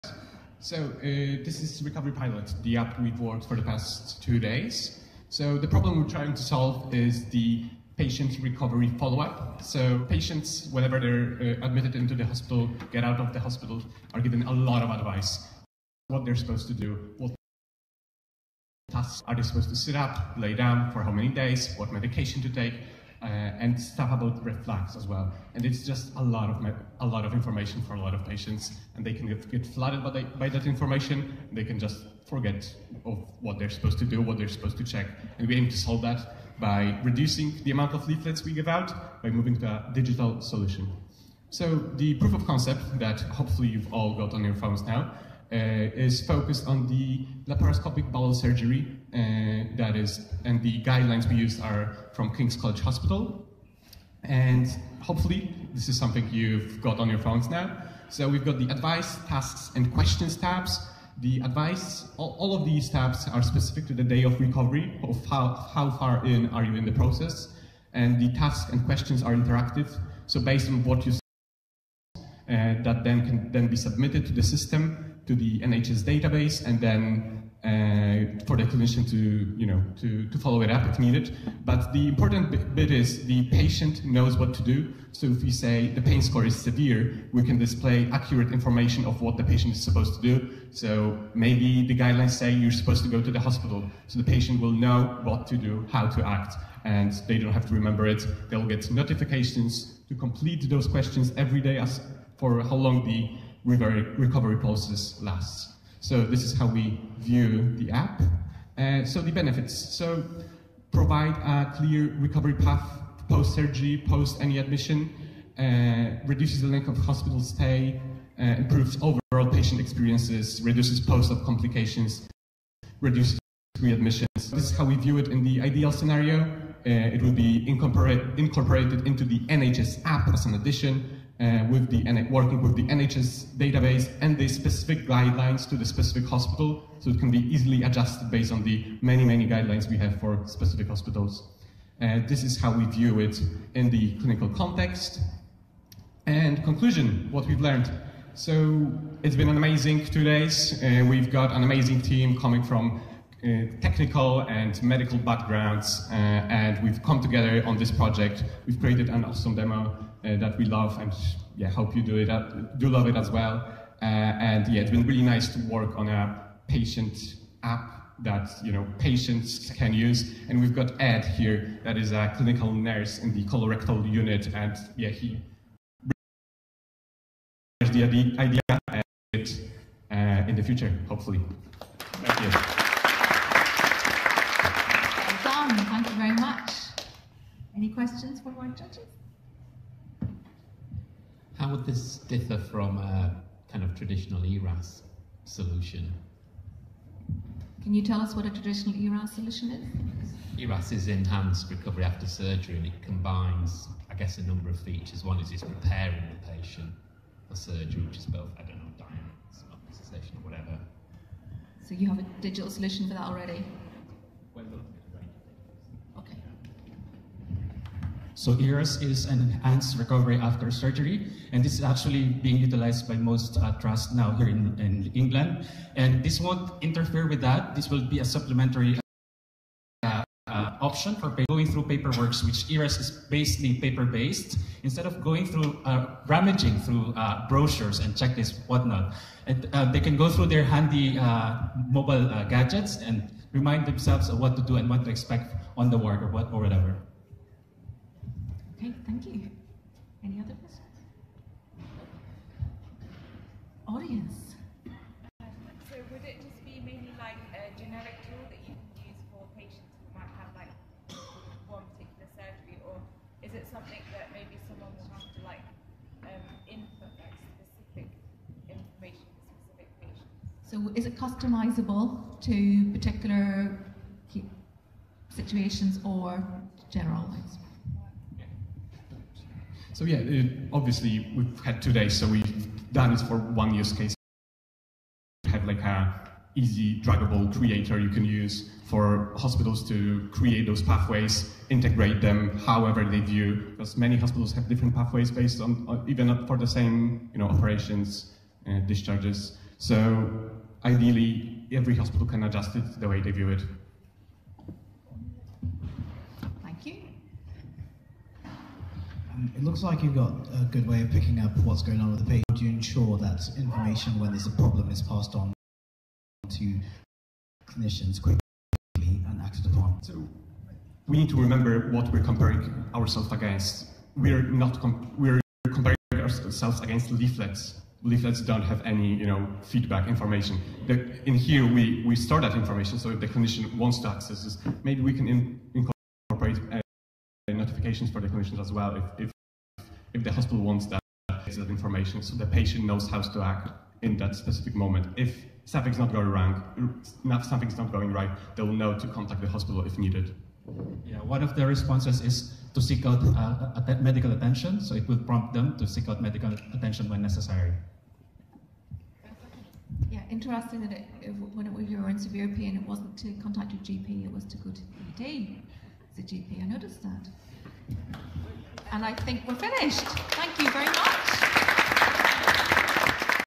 NHS Hack Day #27 Presentations: Recovery Pilot
Recording of the "Recovery Pilot" presentation from NHS Hack Day #27 in London at Chelsea and Westminster Hospital in October 2024.